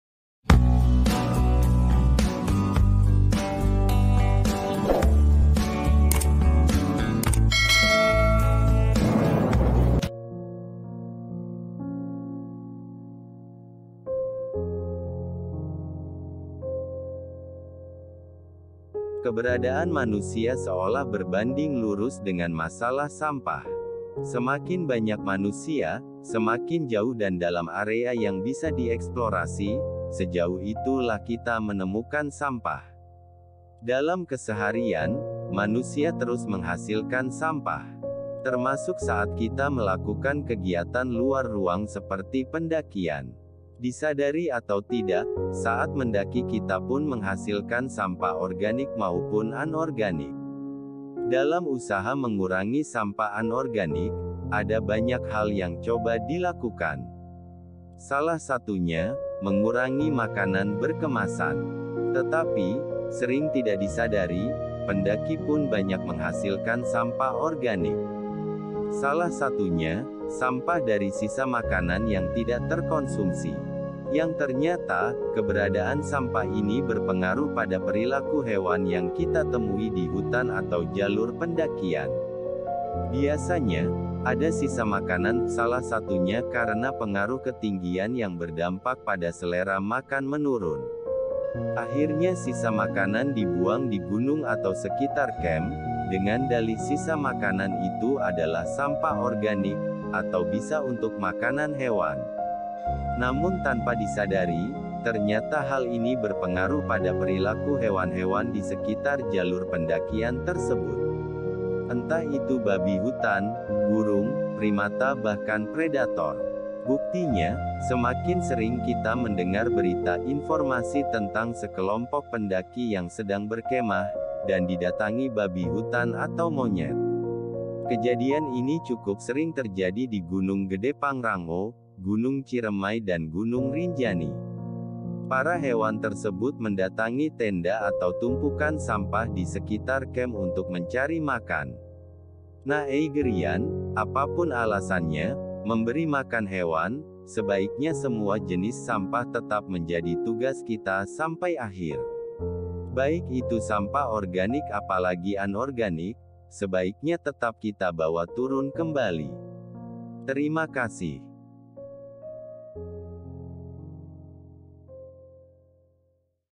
BAHAYANYA MEMBUANG SISA MAKANAN ATAU SAMPAH DI GUNUNG || EIGER KNOWLEDGE
Keberadaan manusia seolah berbanding lurus dengan masalah sampah. Semakin banyak manusia, semakin jauh dan dalam area yang bisa dieksplorasi, sejauh itu lah kita menemukan sampah. Dalam keseharian, manusia terus menghasilkan sampah. Termasuk saat kita melakukan kegiatan luar ruang seperti pendakian. Disadari atau tidak, saat mendaki kita pun menghasilkan sampah organik maupun anorganik.

Dalam usaha mengurangi sampah anorganik, ada banyak hal yang coba dilakukan. Salah satunya, mengurangi makanan berkemasan.  Tetapi, sering tidak disadari, pendaki pun banyak menghasilkan sampah organik. Salah satunya, sampah dari sisa makanan yang tidak terkonsumsi. Yang ternyata, keberadaan sampah ini berpengaruh pada perilaku hewan yang kita temui di hutan atau jalur pendakian.

Biasanya, ada sisa makanan (salah satunya) karena pengaruh ketinggian yang berdampak pada selera makan menurun. Akhirnya sisa makanan dibuang di gunung atau sekitar camp, dengan dalih sisa makanan itu adalah sampah organik, atau bisa untuk makanan hewan.

Namun tanpa disadari, ternyata hal ini berpengaruh pada perilaku hewan-hewan disekitar jalur pendakian tersebut. Entah itu babi hutan, burung, primata bahkan predator.  Buktinya, semakin sering kita mendengar berita/ informasi tentang sekelompok pendaki yang sedang berkemah, dan didatangi babi hutan atau monyet.

Kejadian ini cukup sering terjadi di Gn. Gede Pangrango, Gn. Ciremai dan Gn. Rinjani. Para hewan tersebut mendatangi tenda atau tumpukan sampah di sekitar camp untuk mencari makan.

Nah Eigerian, apapun alasannya, memberi makan hewan, sebaiknya semua jenis sampah tetap menjadi tugas kita sampai akhir ya. Baik itu sampah organik apalagi anorganik, sebaiknya tetap kita bawa turun kembali.